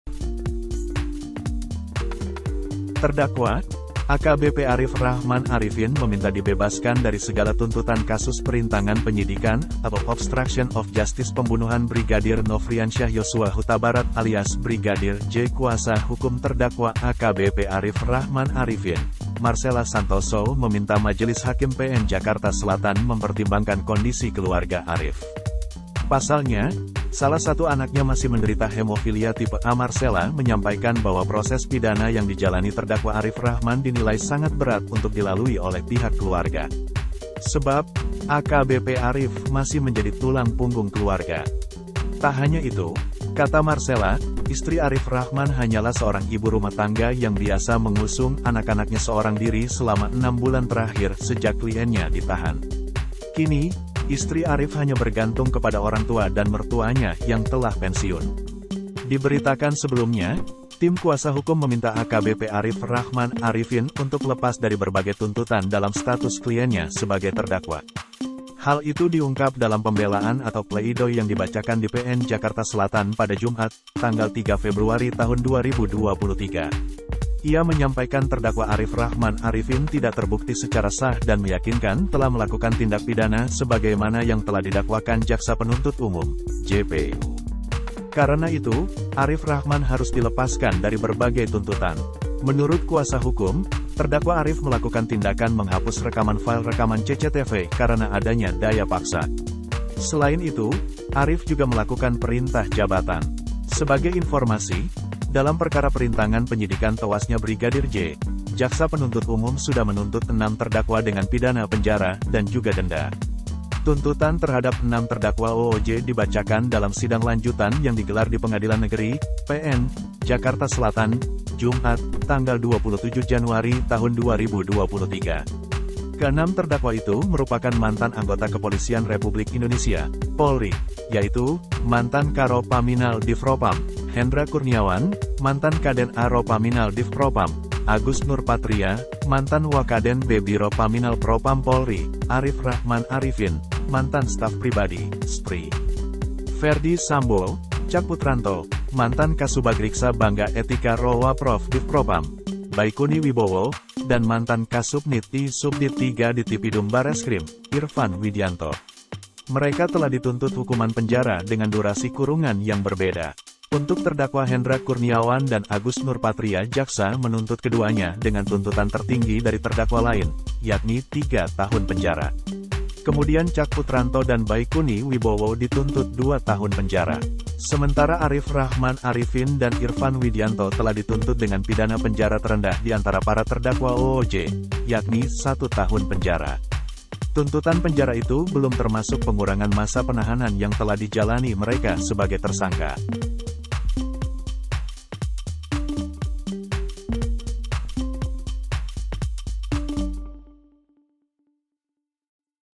AKBP Arif Rachman Minta Dibebaskan, Punya Anak yang Masih Butuh Biaya Pengobatan
Terdakwa, AKBP Arif Rachman Arifin meminta dibebaskan dari segala tuntutan kasus perintangan penyidikan atau obstruction of justice pembunuhan Brigadir Nofriansyah Yosua Hutabarat alias Brigadir J.

Kuasa hukum terdakwa AKBP Arif Rahman Arifin, Marcella Santoso meminta Majelis Hakim PN Jakarta Selatan mempertimbangkan kondisi keluarga Arif.

Pasalnya, salah satu anaknya masih menderita hemofilia tipe A.

Editor: Rio